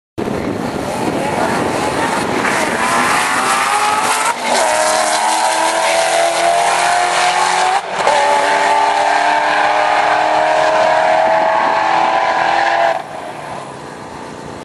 VIPER VS SHELBY GT 500
Rolling with the Viper SNAKERACERX manages the win !